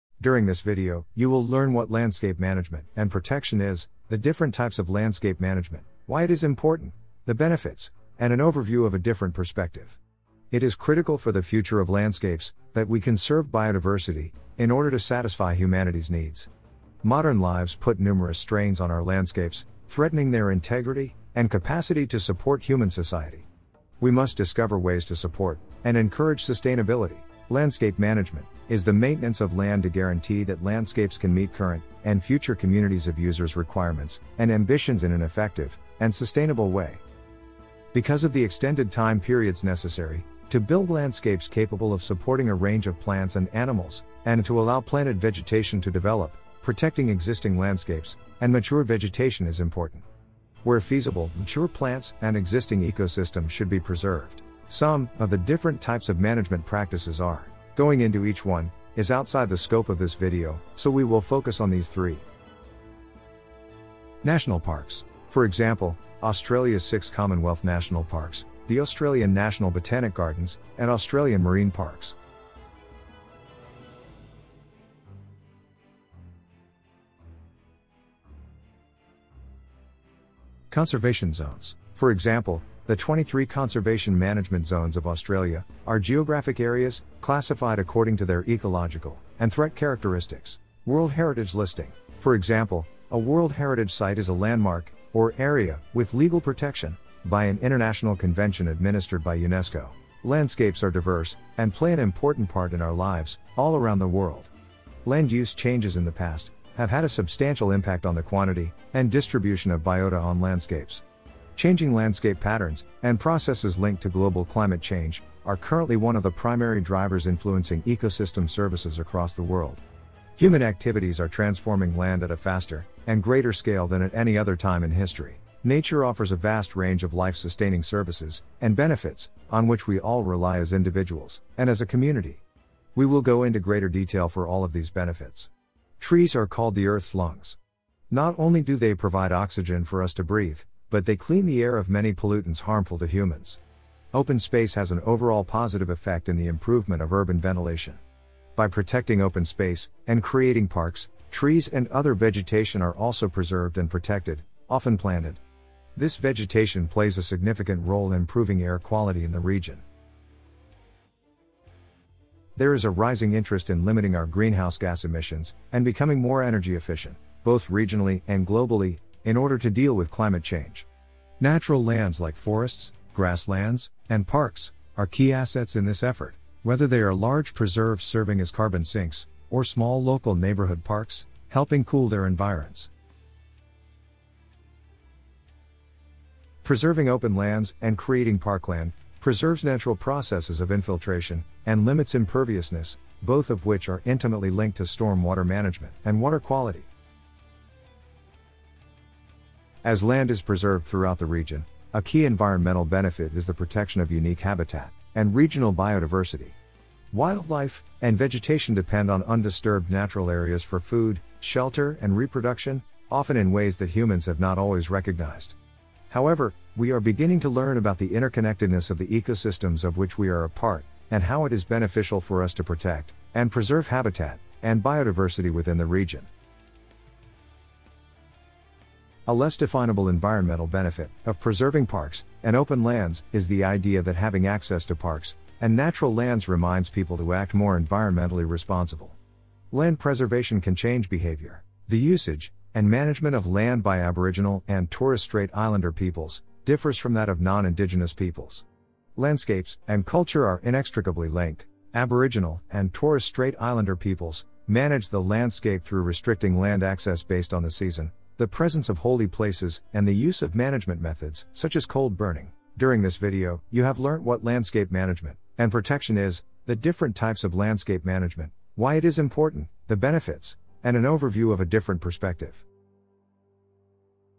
Landscape management and protection
Investigate ways people, including Aboriginal and Torres Strait Islander Peoples, manage and protect landscapes.

Video will provide:

Background information for landscape management
What is landscape management
Types of landscape management
Why is landscape management important
Benefits of landscape management
- Improved Air Quality
- Climate Change
- Improved Water Quality
- Biodiversity and Habitat Protection
- People Act Greener
Aboriginal and Torres Strait Islander perspective